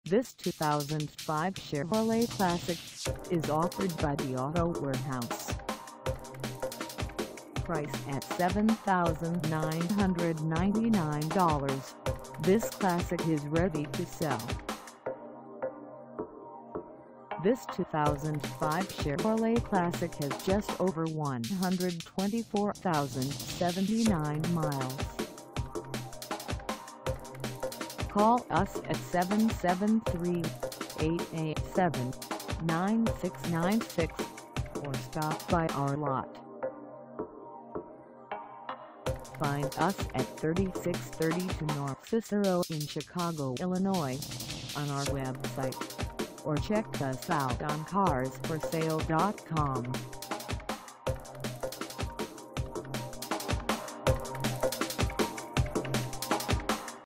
2005 Chevrolet Classic - for sale in Chicago, IL 60641
The Auto Warehouse
3632 North Cicero

Come test drive this 2005 Chevrolet Classic  for sale in Chicago, IL.

Air Conditioning1Power Steering1Power Door Locks1Power Windows1Tachometer1Tilt Steering Wheel1Cruise Control1Bucket Seats1Daytime Running Lights1Remote Trunk Release1Center Console1Power Mirrors1Rear Window Defroster1Dual Air Bags1Intermittent Wipers1Auto Headlight On/Off